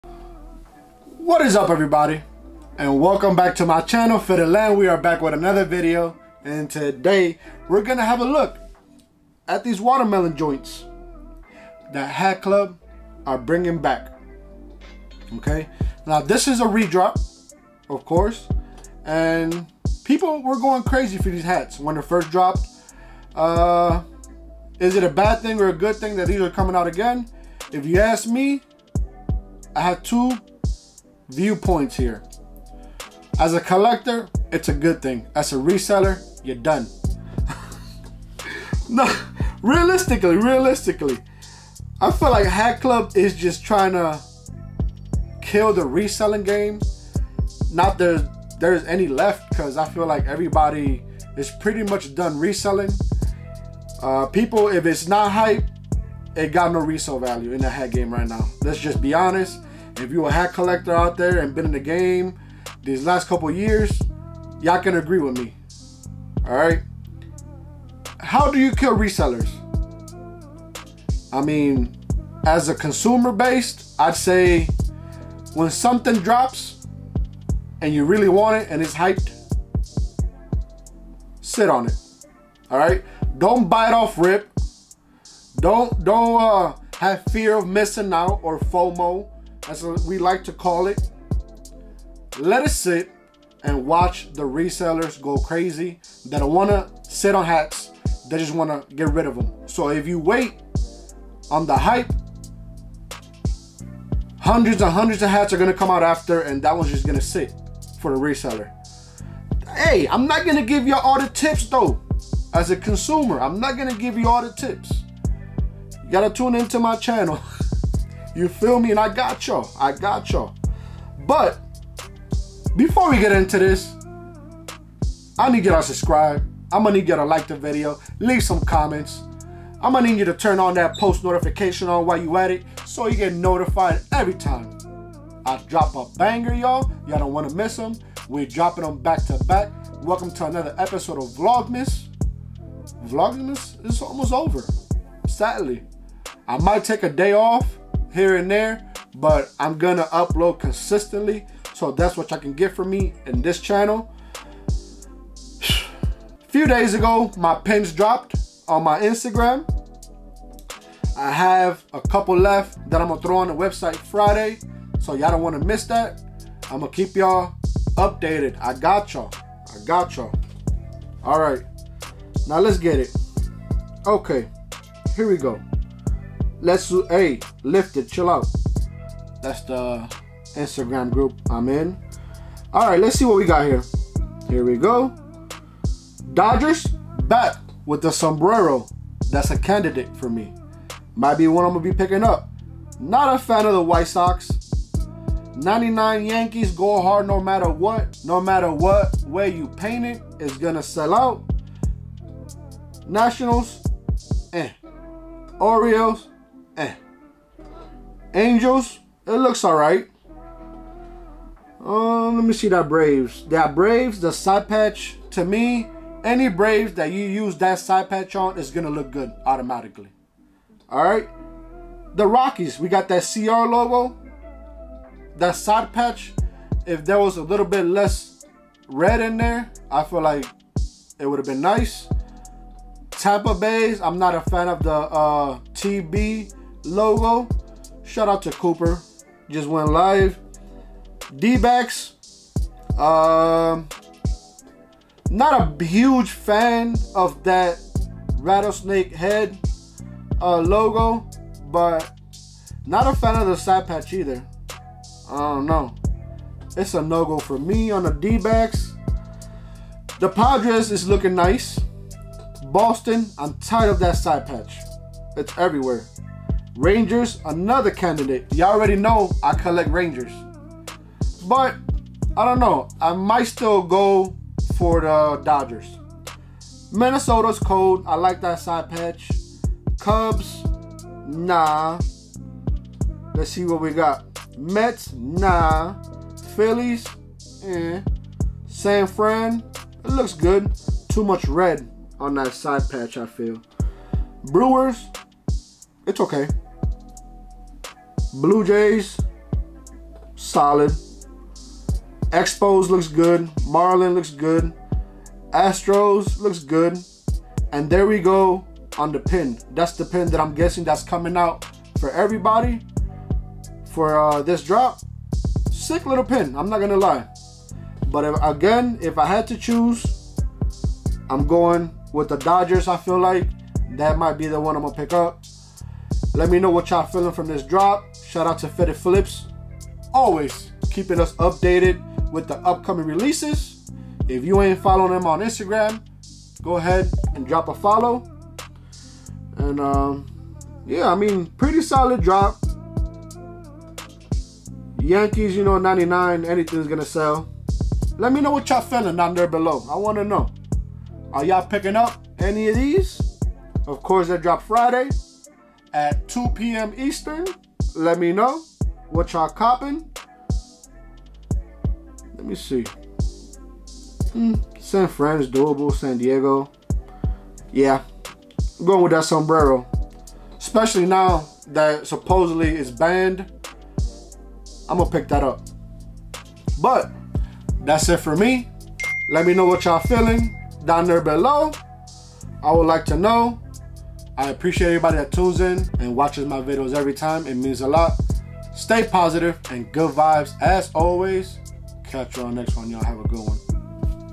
HatClub Dropping The Watermelon Collection Again | Let’s Have A Look!!
Solid collection dropping once again some might say this is a summer hat 🤷🏻‍♂️

Camera i use : Sony ZV-1 Camera for Content Creators, Vlogging and YouTube with Flip Screen and Microphone